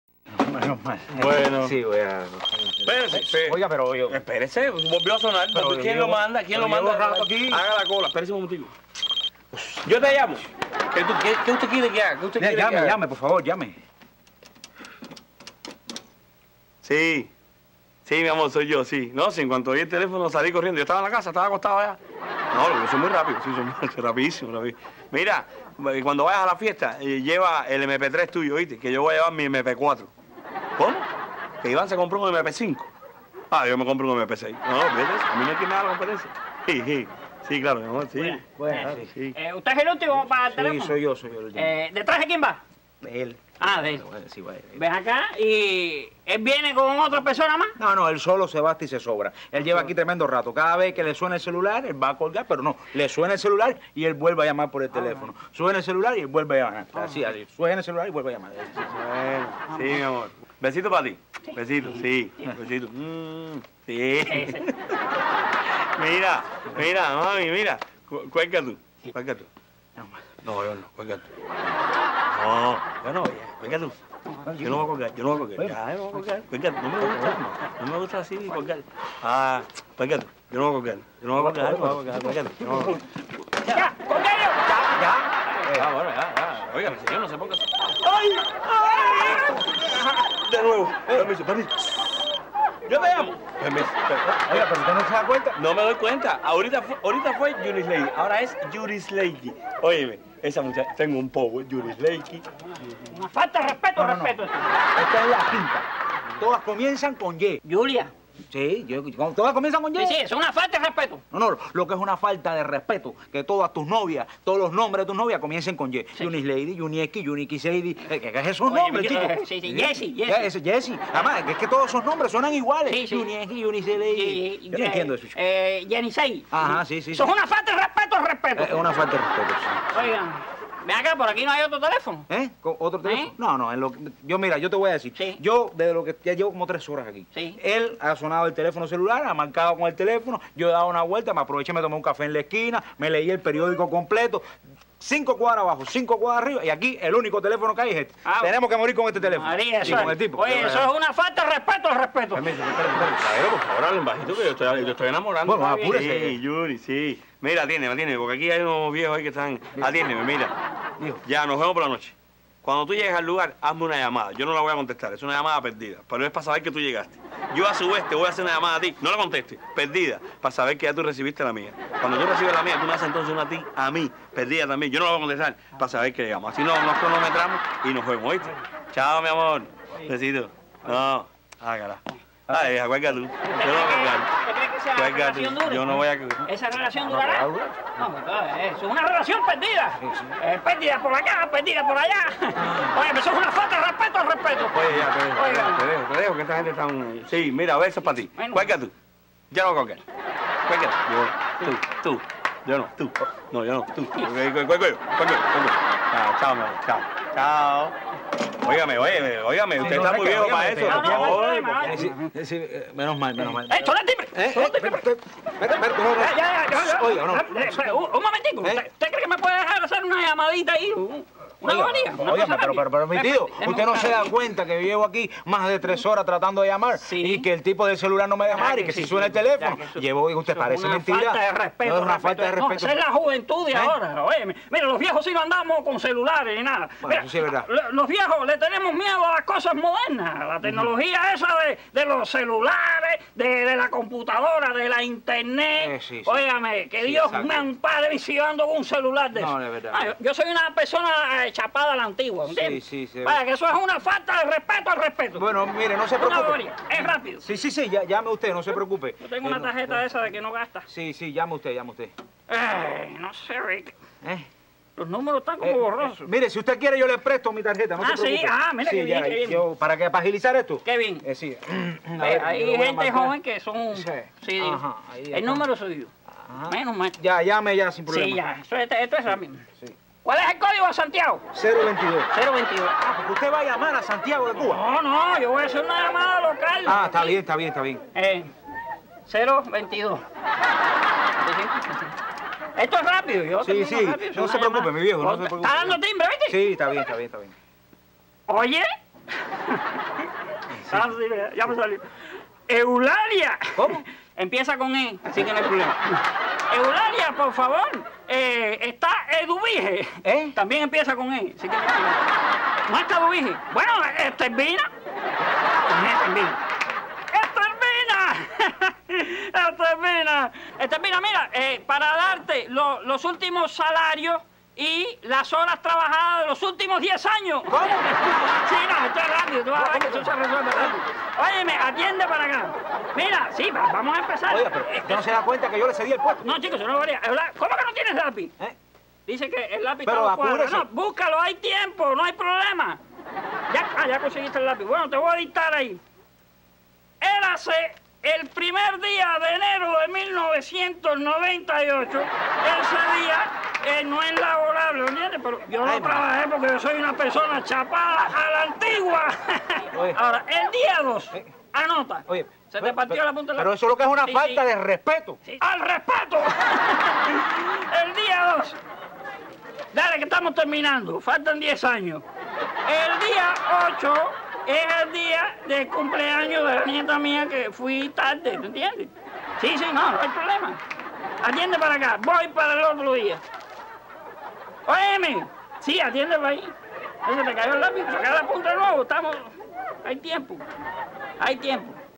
Como hablar por celular publico en Cuba mientras Pipe y Papo esperan por su turno!!!
Como Otto Ortiz habla por celular publico con sus novias mientras Pipe y Papo esperan por su turno!!!